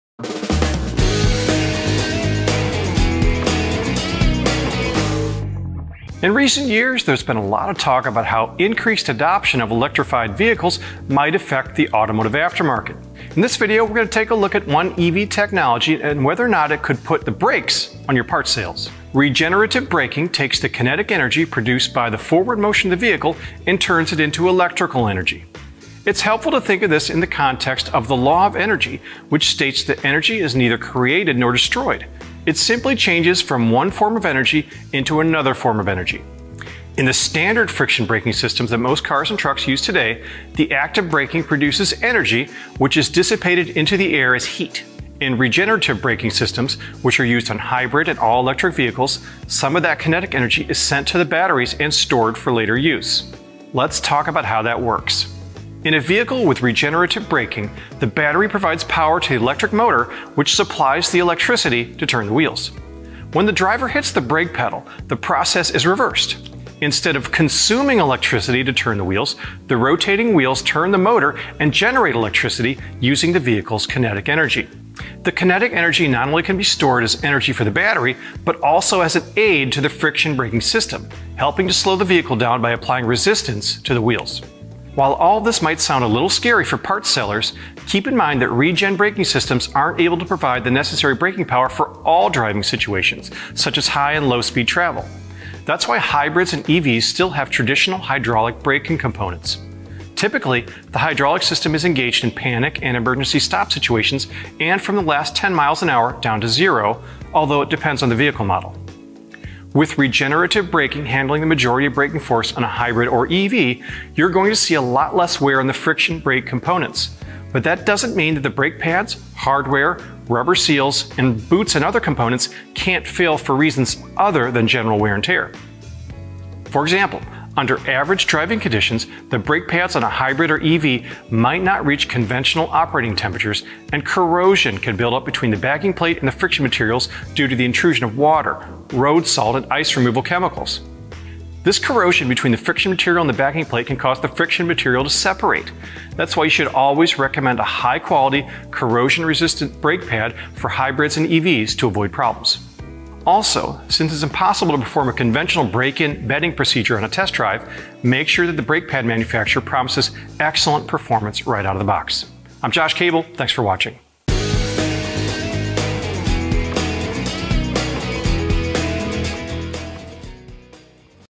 Understanding Regenerative Braking
Regenerative braking takes the kinetic energy produced by the forward motion of the vehicle and turns it into electrical energy. Could this technology put the brakes on your parts sales?